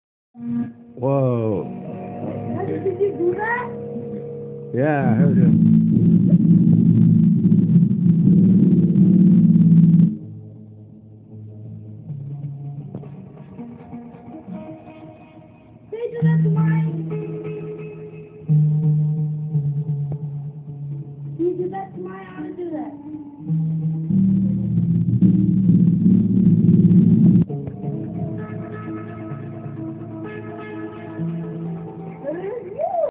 Kids having fun learning & playing guitar
My son Eathen is teaching the neighborhood kids how to play guitar